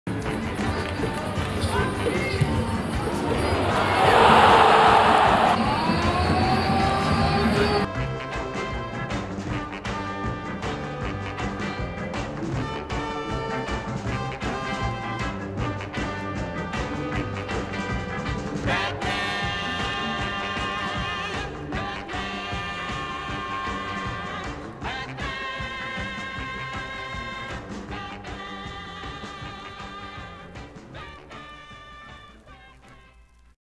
Trailer - É para avançar!! (Arouca VS Sporting)
O Sporting joga amanhã para a 10ªJornada da Liga NOS frente ao Arouca. Depois da derrota diante do Skenderbeu, a equipa de Jorge Jesus está obrigado a vencer para continuar na liderança. É há uma boa noticia, em que Madjer foi o eleito para o melhor jogador do mundo em Futebol de Praia, depois de ter vencido o Mundial em Espinho. Uma grande noticia!